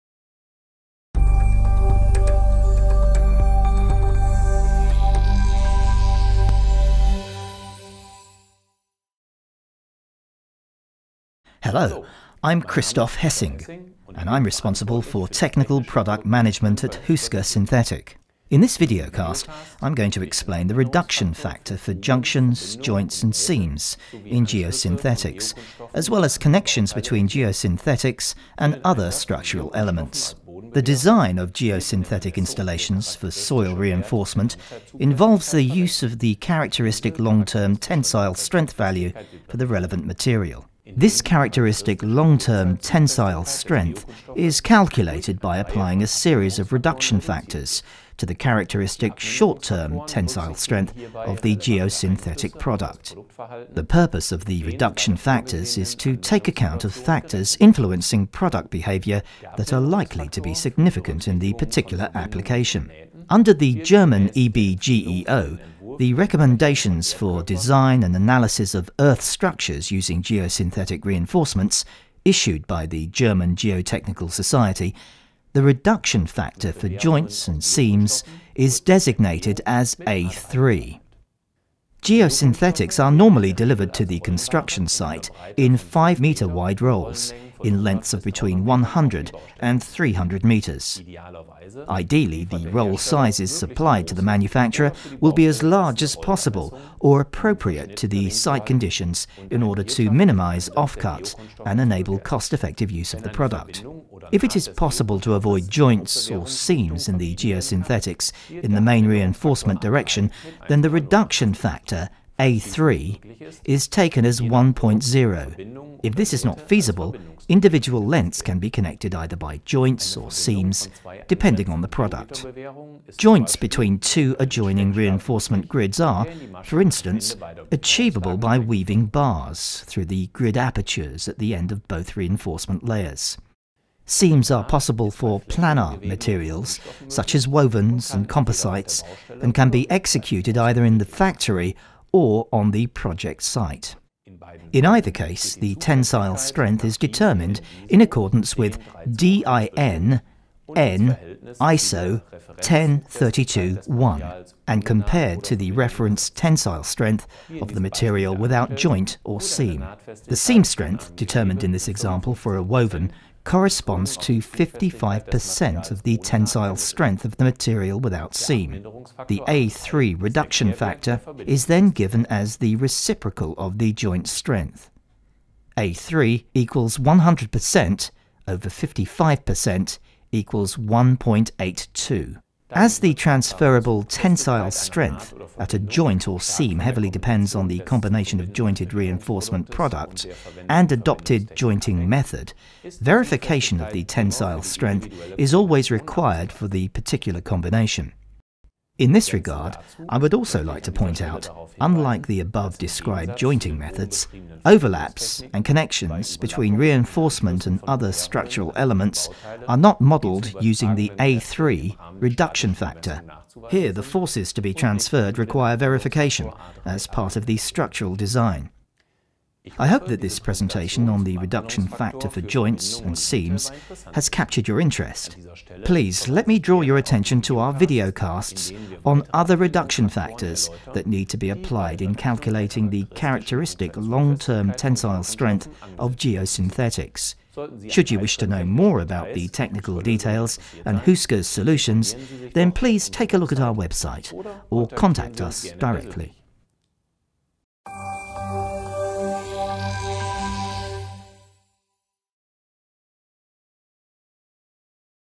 VideoCast | Reduction Factor A3 - Joints
In this video we explain the reduction factor for junctions, joints and seams in geosynthetics as well as connections between geosynthetics and other structural elements.